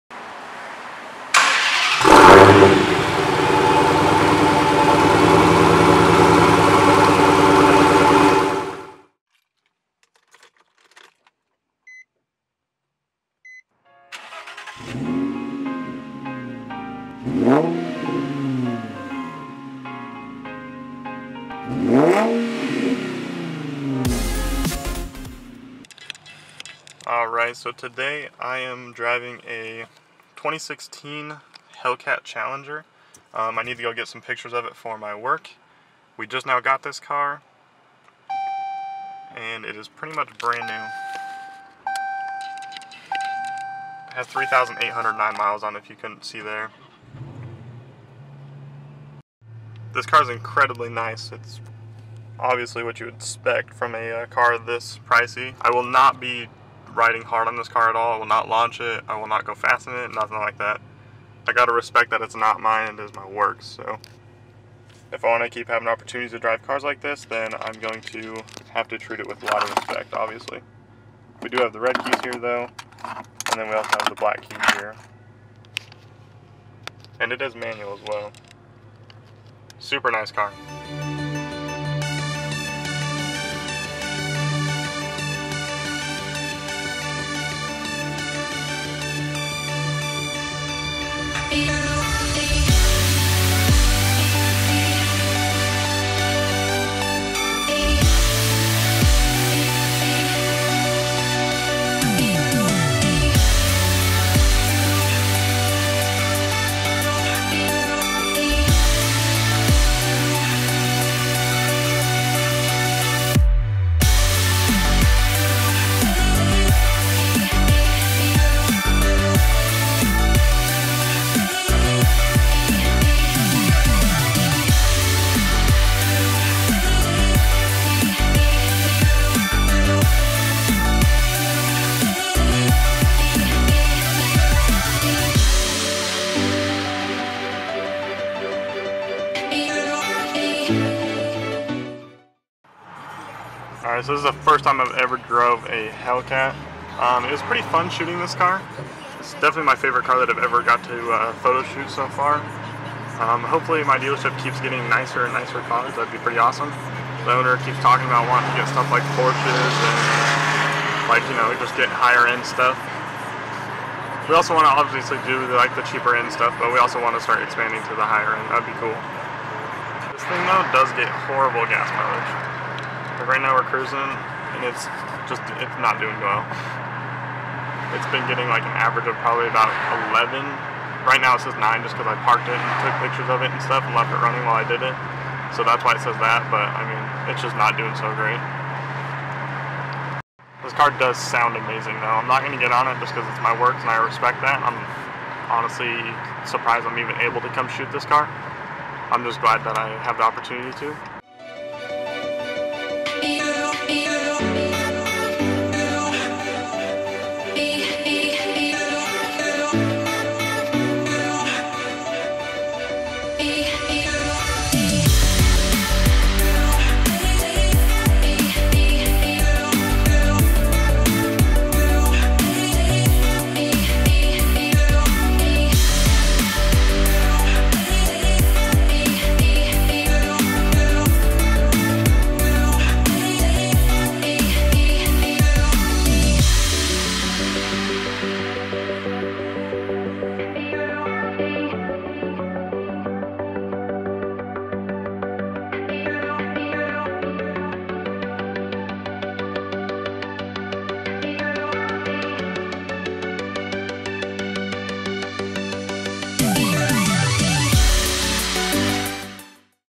Dodge Challenger Hellcat Review/Short Film
My first time driving a Hellcat, or even a challenger for that matter. Also the highest horsepower car i've ever driven. This is my works car, therefore iw ill not be driving it recklessly, as much as i wish i could! THIS CAR IS FOR SALE FOR 59,998! Comment if interested! We can ship it to you!